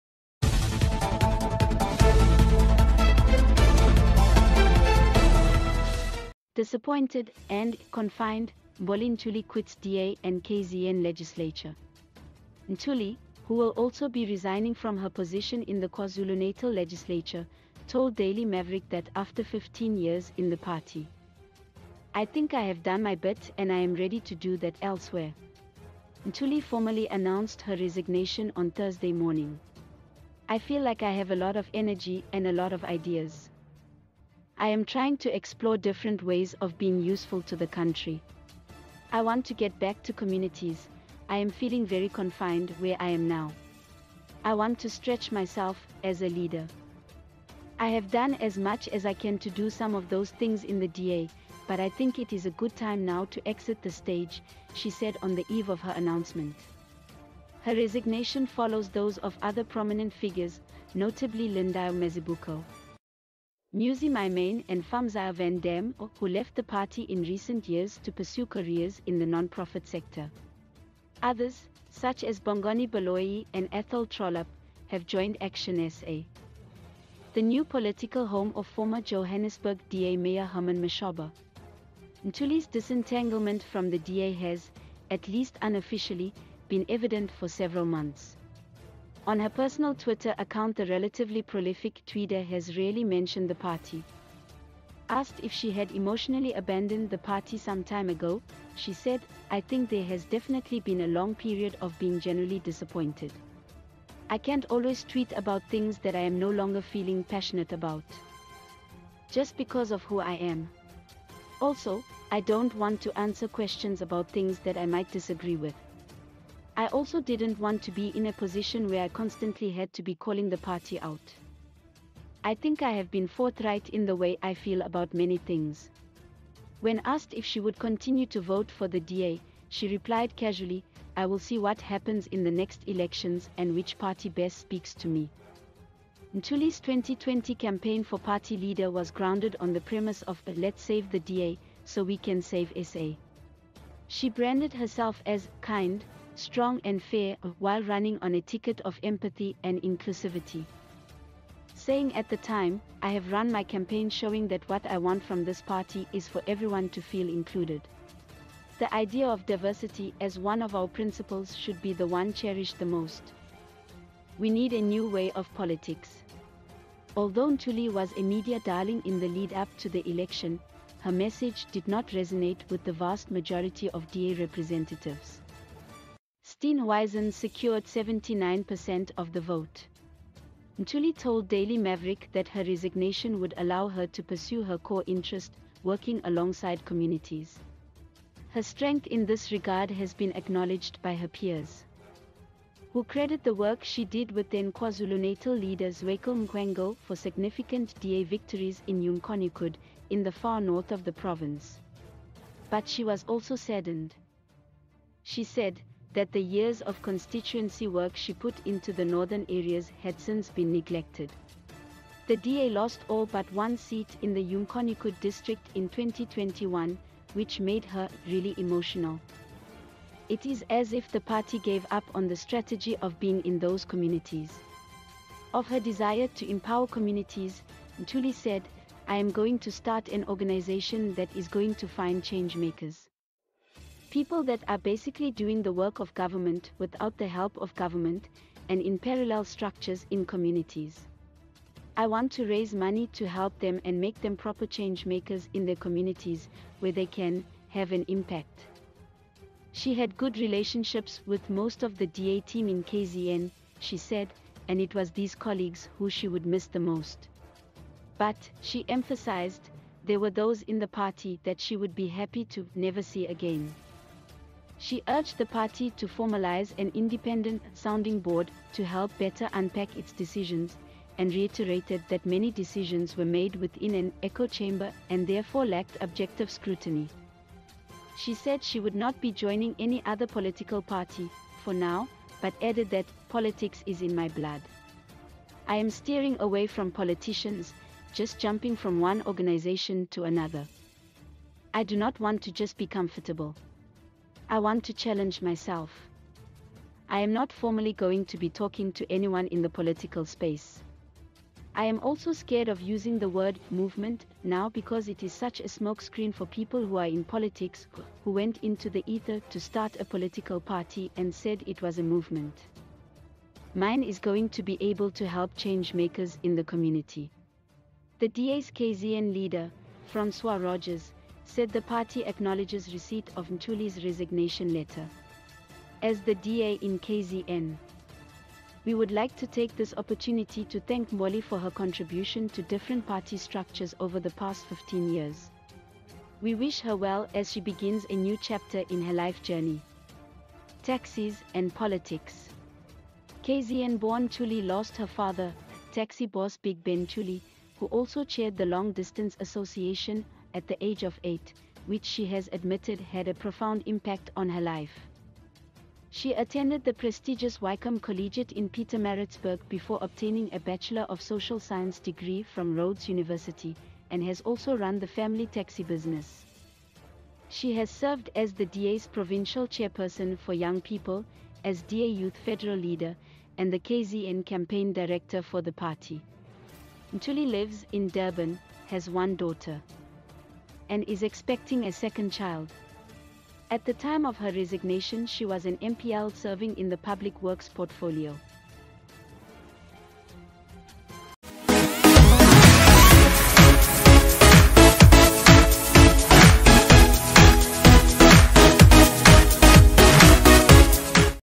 Mbali Ntuli quits DA and KZN legislature
Ntuli, who will also be resigning from her position in the KwaZulu-Natal legislature, told Daily Maverick that after 15 years in the party, “I think I have done my bit and I am ready to do that elsewhere”.

Ntuli formally announced her resignation on Thursday morning.

▬▬▬▬▬▬ஜ۩۞۩ஜ▬▬▬▬▬▬
🎥 MAY YOU WILL LIKE:

▬▬▬▬▬▬ஜ۩۞۩ஜ▬▬▬▬▬▬
🎥 MORE VIDEO BIOGRAPHY SOUTH AFRICA: